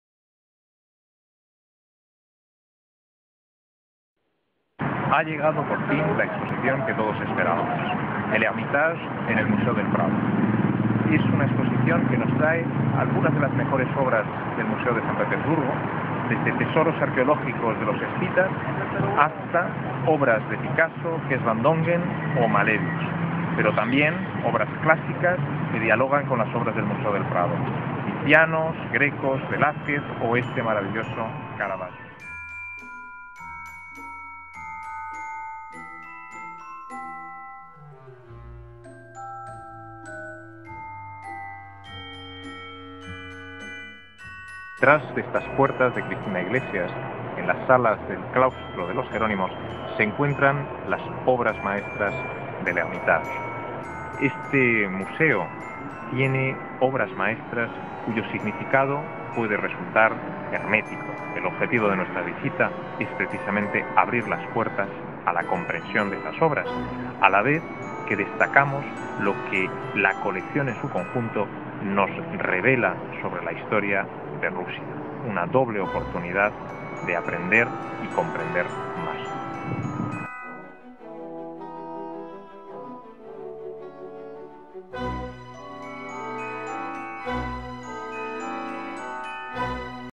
Exposición "El Hermitage en el Museo del Prado"
Madrid Original propone una visita guiada privada a la exposición "El Hermitage en el Prado".